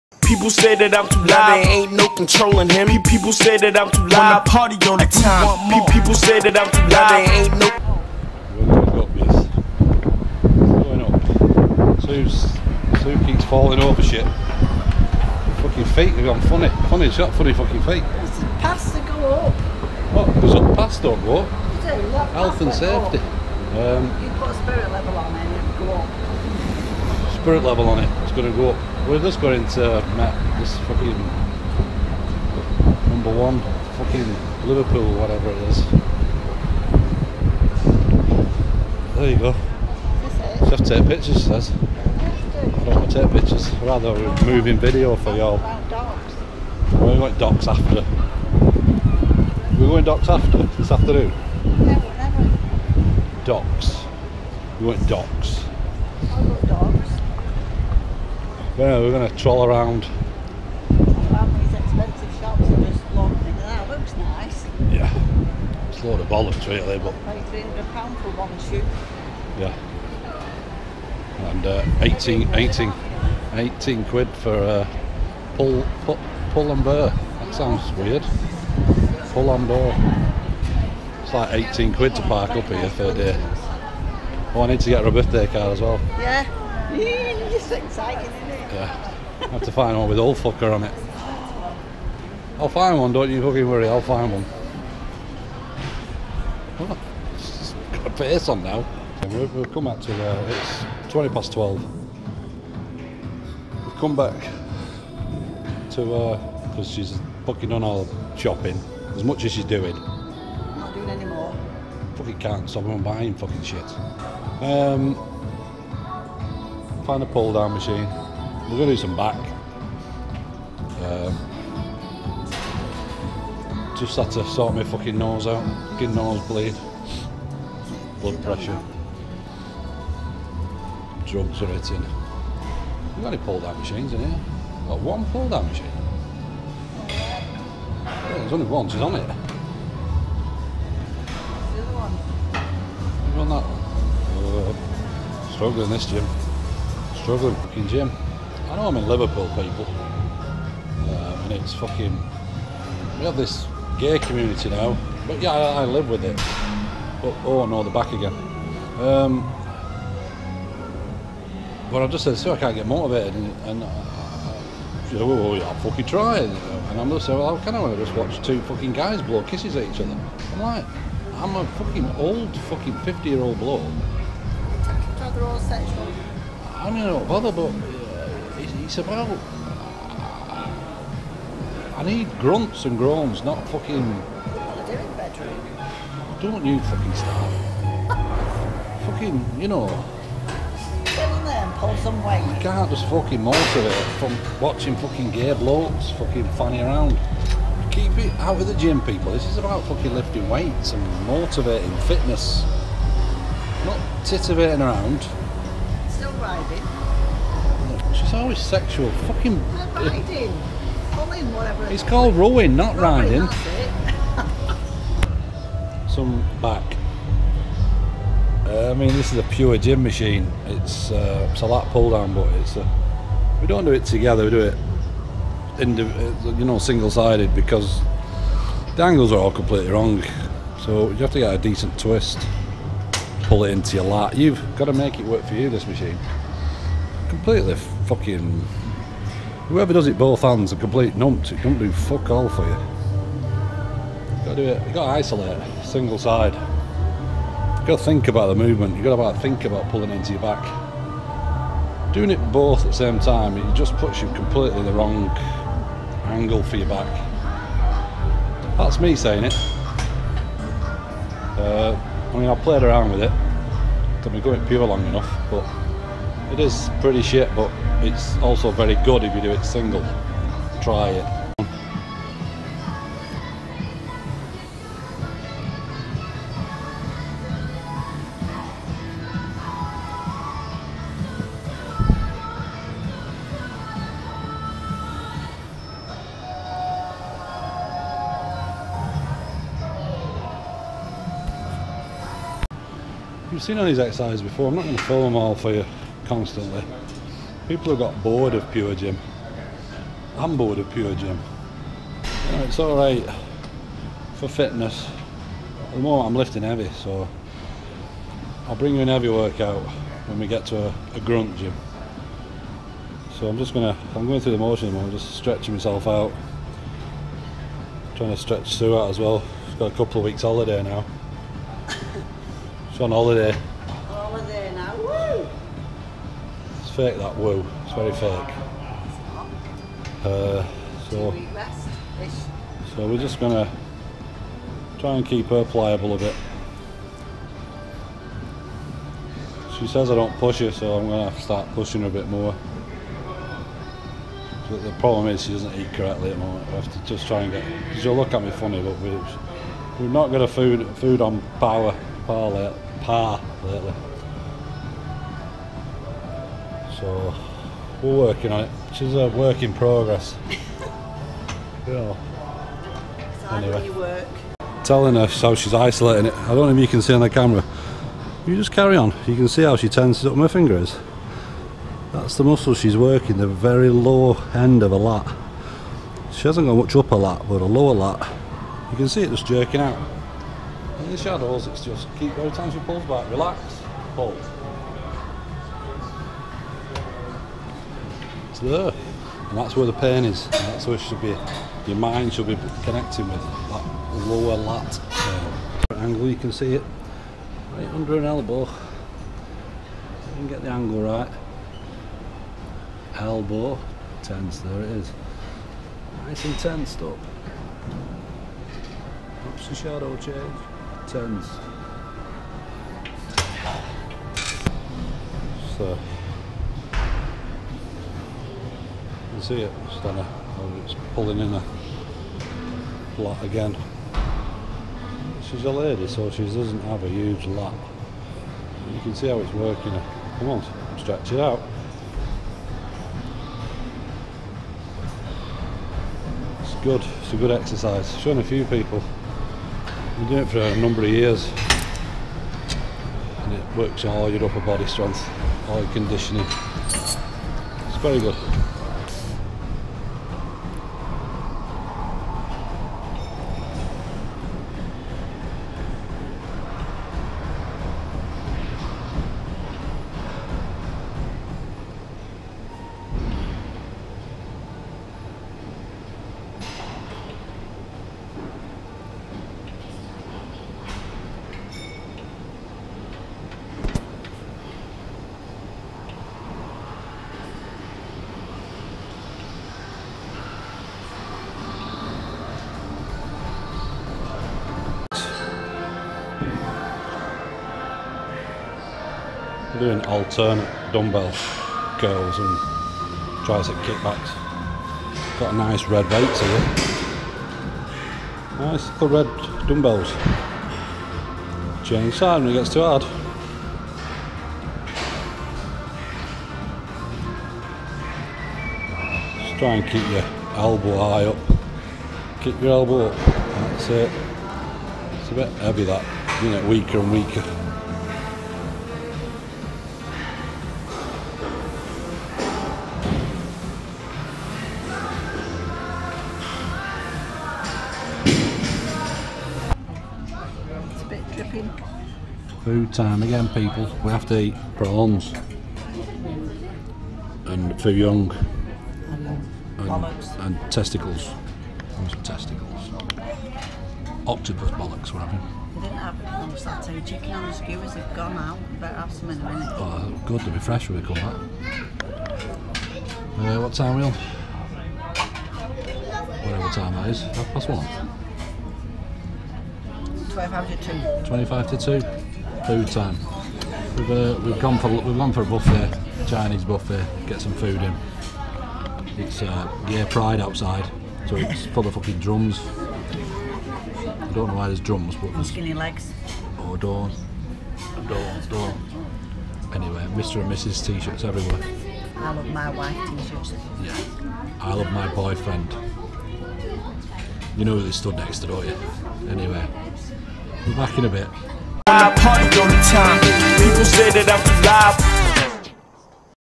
Saturday 30th July 2nd gym session
Got to the Shops for a few Hours, back to the Gym Little Workout then Food.

Follow and Comment me on